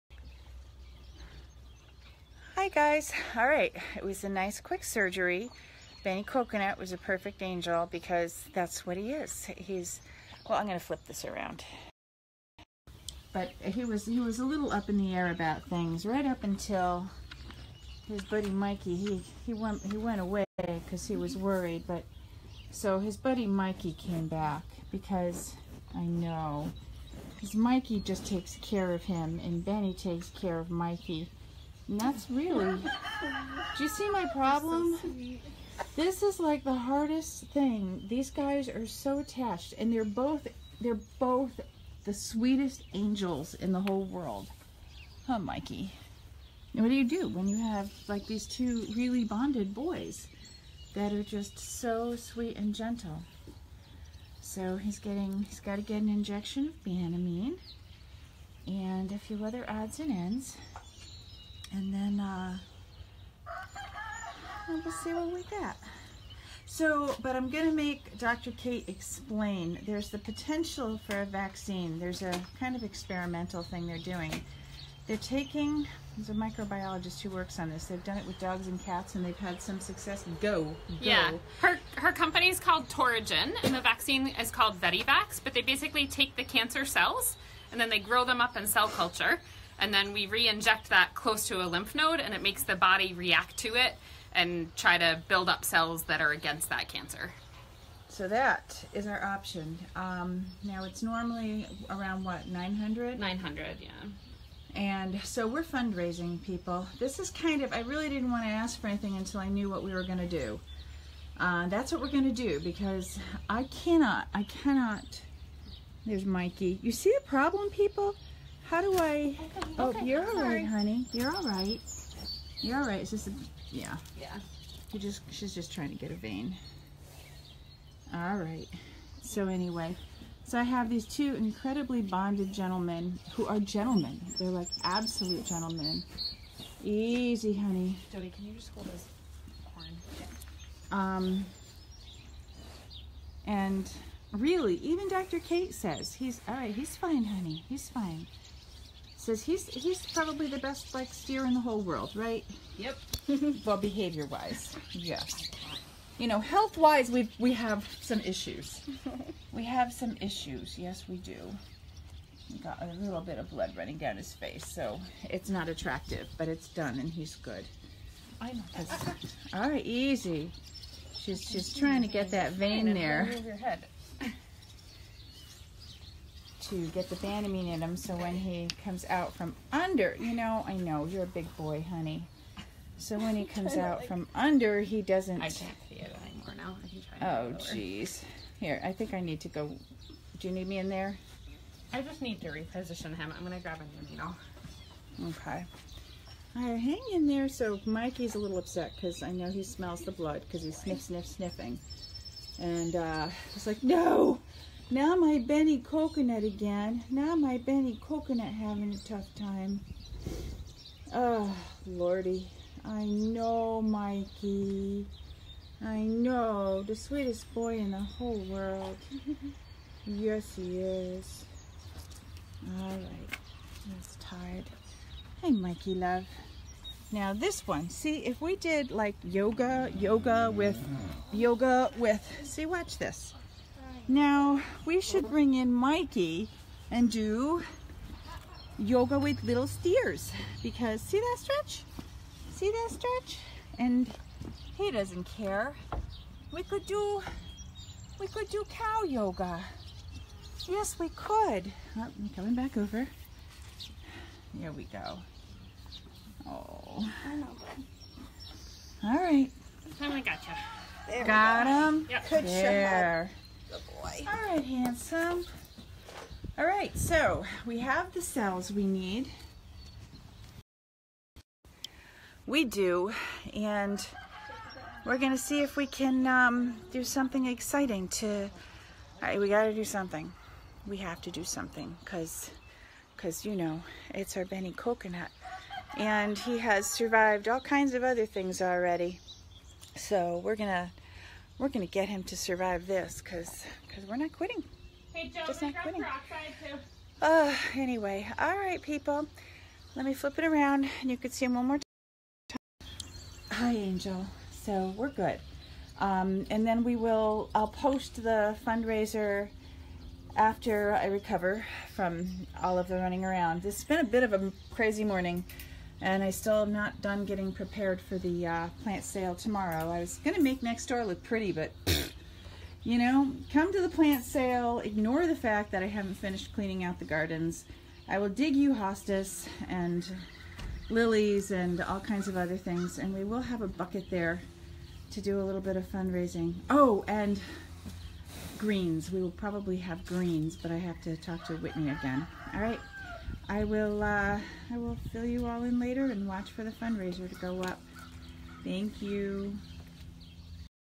Benny Coconut after his eye surgery
Benny Coconut's eye cancer is back after he already lost his other eye to it two years ago. We are raising money to try an experimental vaccine to see if the sweetest steer in the entire world can recover and stay with us for at least a few years longer.